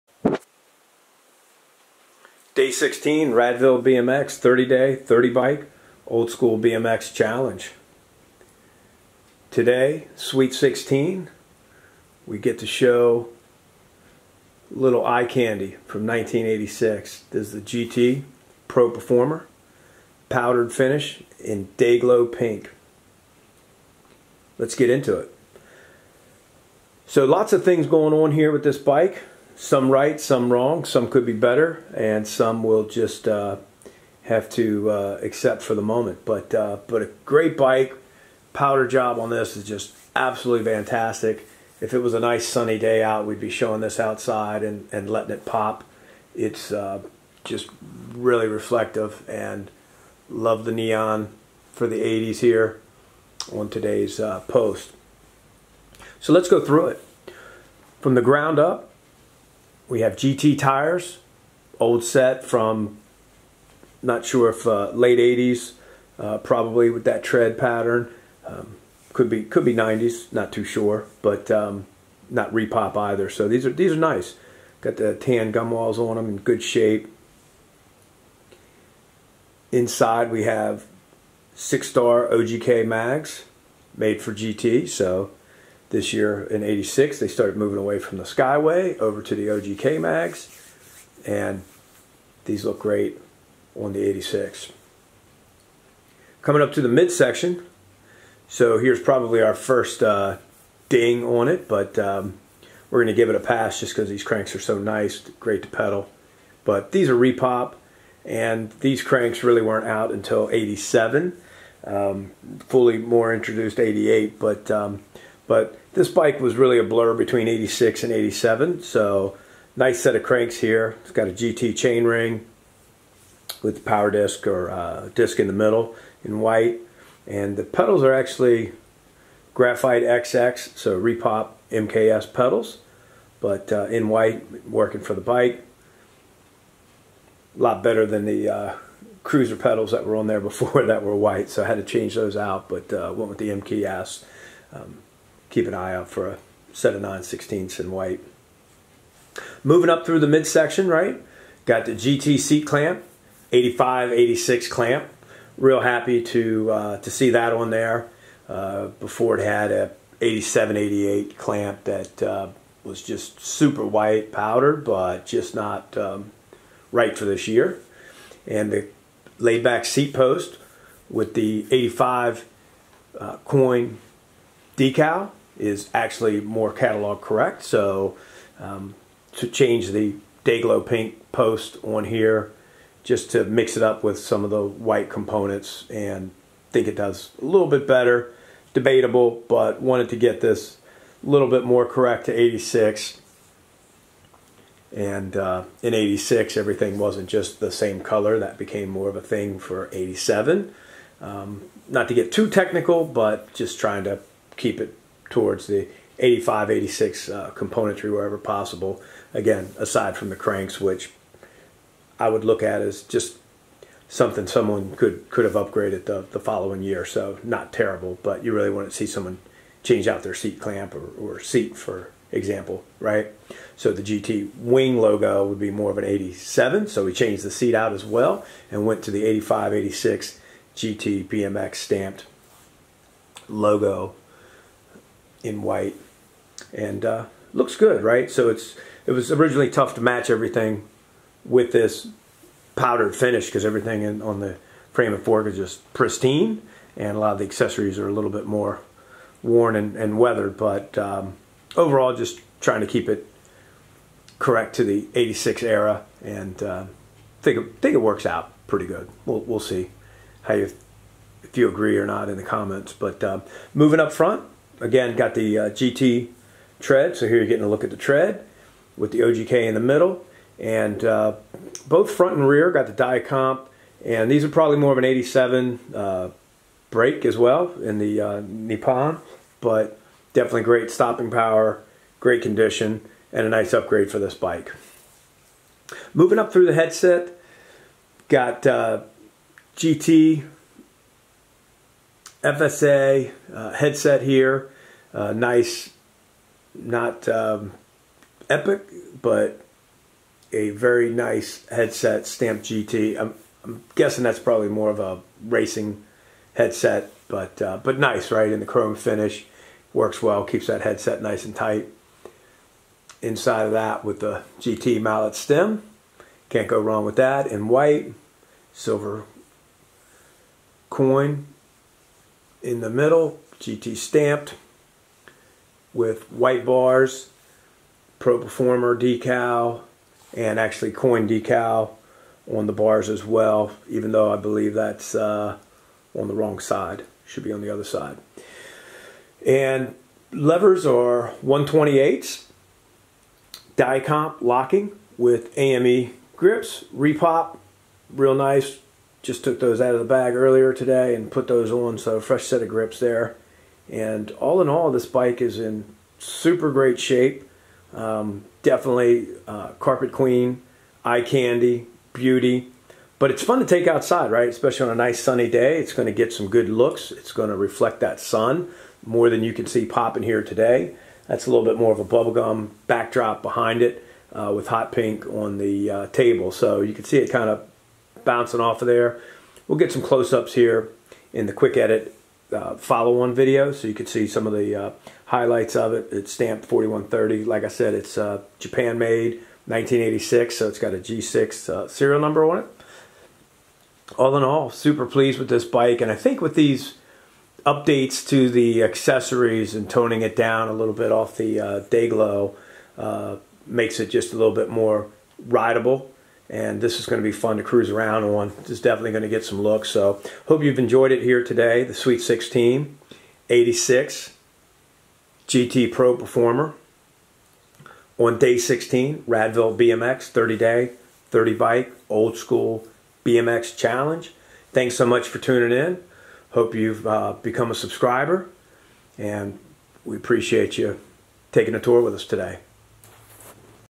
Day 16 Day Glo Pink 1986 GT Pro Performer
1986 GT Pro Performer powered Day Glo Pink, GT tires, OGK mags, 3 piece GT crankset repop, GT chainring 44T, MKS Graphite XX repop, GT coin seat post, GT bars, GT mallet stem, dua compe brakes front and rear with 128 levers, AME grips, dice valve caps, GT seat and clamp